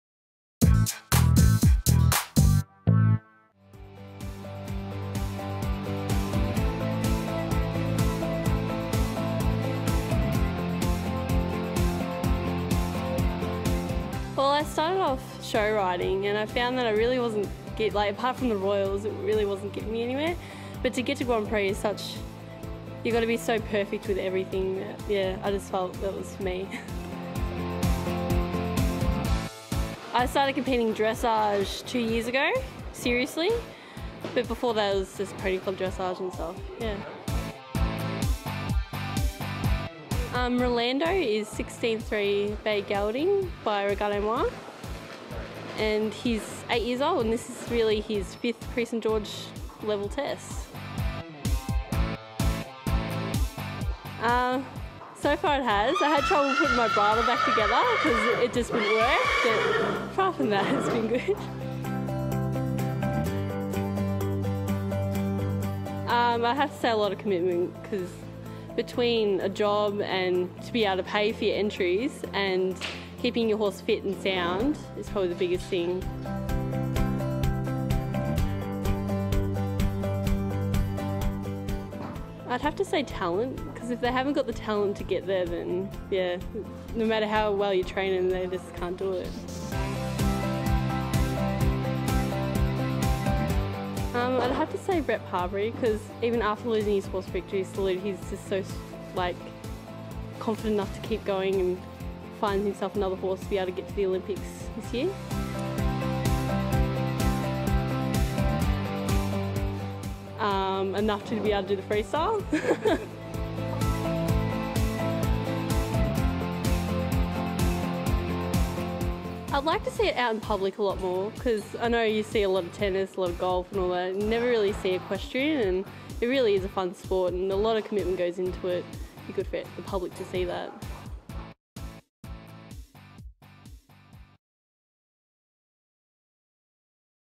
Top Horse TV | Emma Crocker Interview
Emma Crocker Interview
Video by Top Horse -- dressage horses for sale
Top Horse TV - Up and coming dressage rider Emma Crocker has a chat with Top Horse.